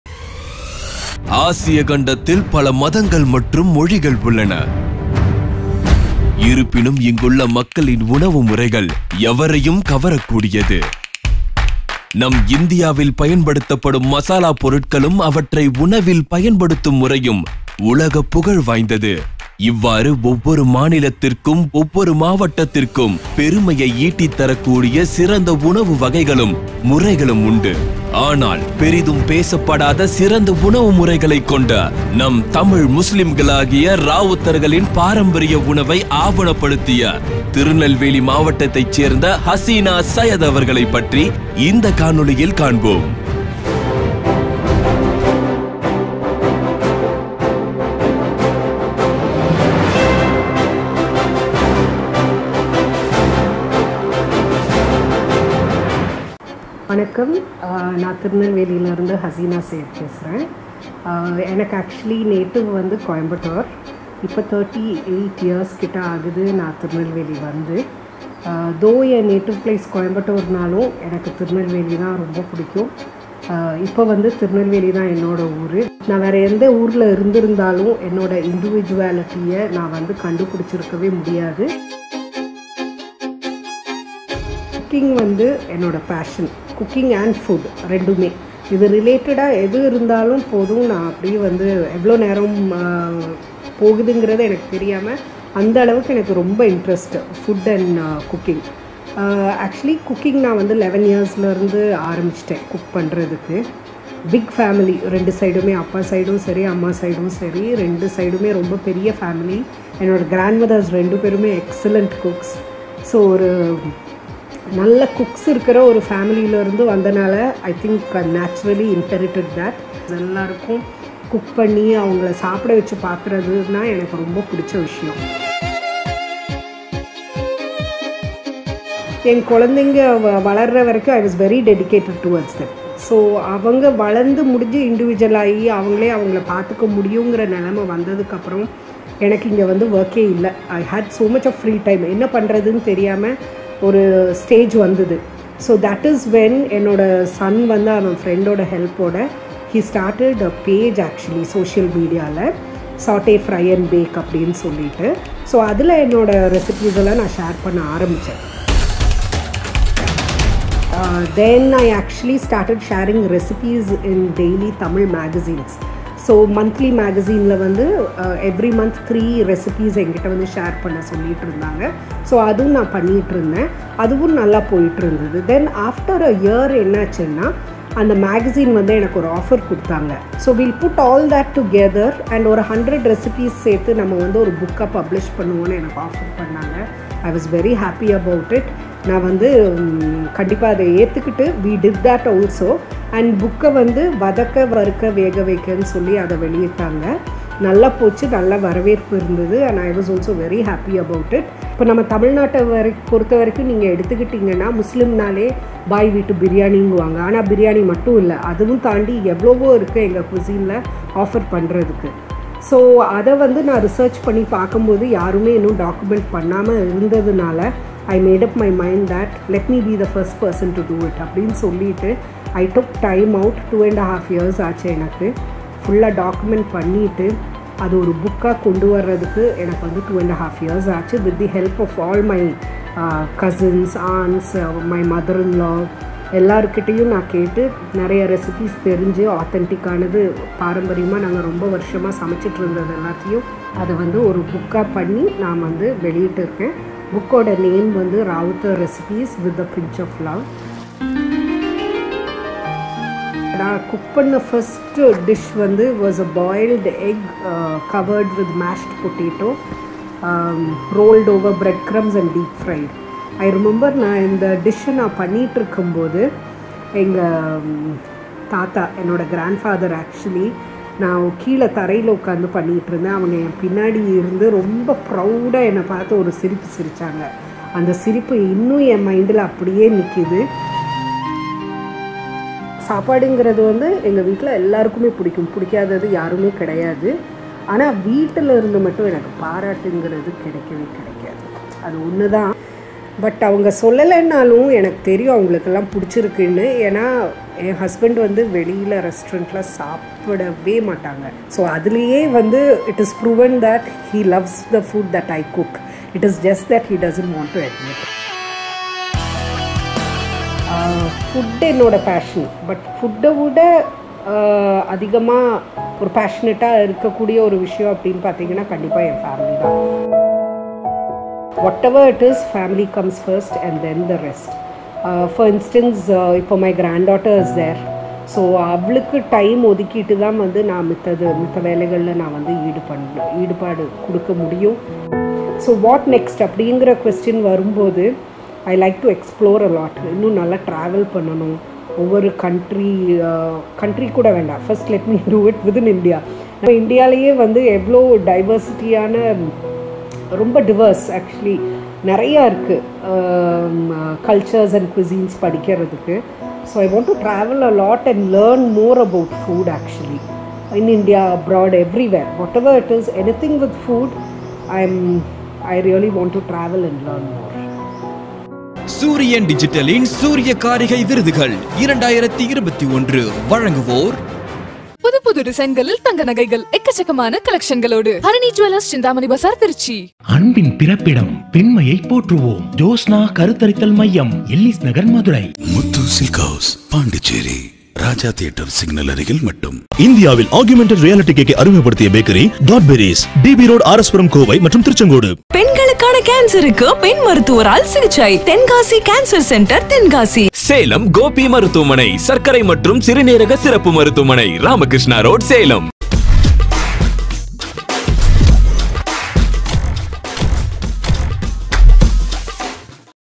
திருநெல்வேலியின் சூரியக்காரிகை ஹசீனா செய்யது | சூரியக்காரிகை விருதுகள் 2021
பாரம்பரிய உணவுகளை அடையாளப்படுத்தி தமிழர்களின் உணவுமுறைகளை உலகிற்கு ஆவணப்படுத்திய திருநெல்வேலியின் சூரியக்காரிகை ஹசீனா செய்யது.